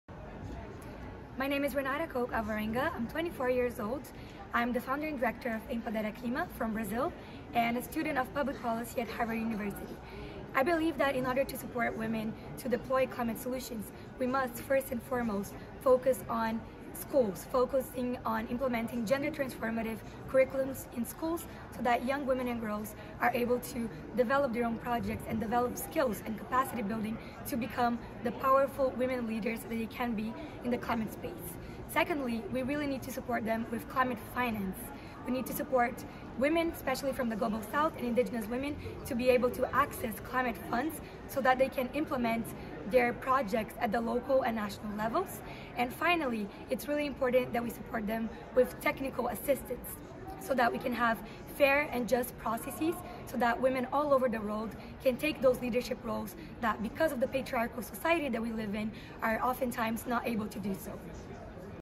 Renata Koch Alvarenga Discusses Women Leading on Climate
Renata highlights the support necessary for womeninclimate such as
1) Gender transformative curriculums in school
2) Climate finance
3) Technical assistance

Listen to her and share your video on the importance of womenleadingonclimate